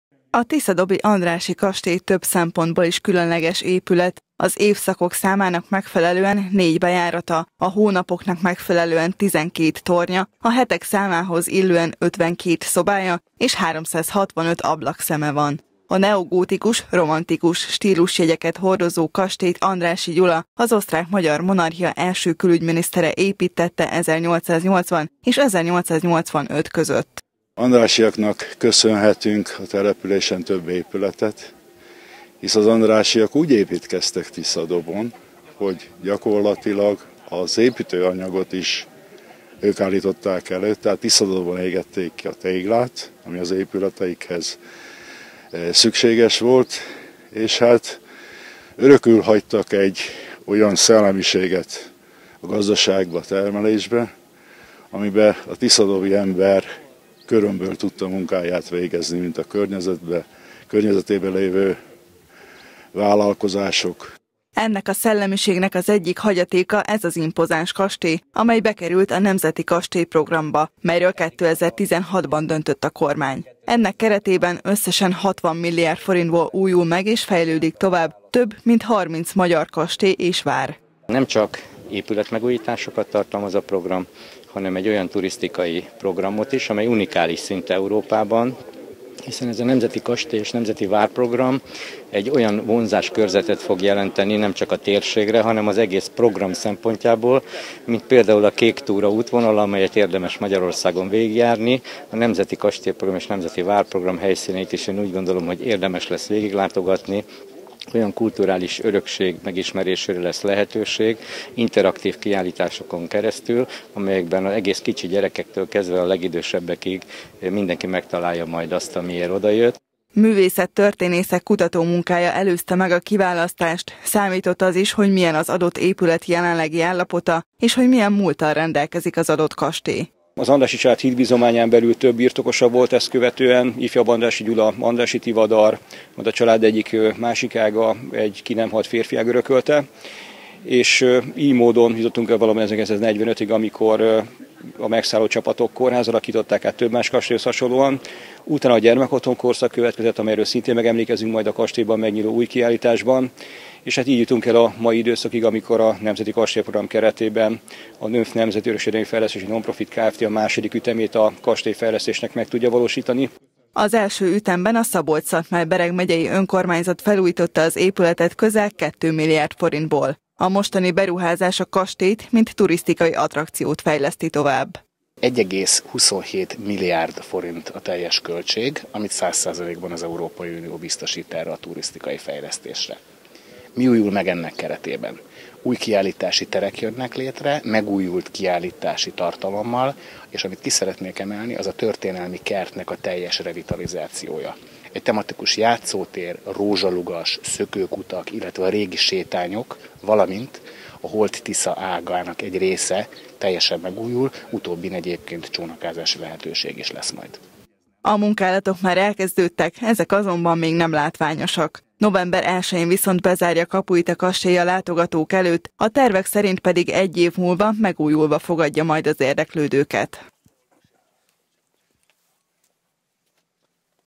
Turisztikai fejlesztés az Andrássy-kastélyban - Kölcsey Tv
Közel 1,3 milliárd forintból többek között tematikus játszóteret és rózsalugast is kialakítanak a tiszadobi Andrássy-kastély történelmi kertjében. Emellett új állandó kiállítás költözik az épületbe és az éttermet is korszerüsíteik. A teljes beruházás a Nemzeti Kastélyprogram és Nemzeti Várprogram keretében valósul meg. A munkálatok már elkezdődtek, és a tervek szerin egy év múlva be is fejeződnek.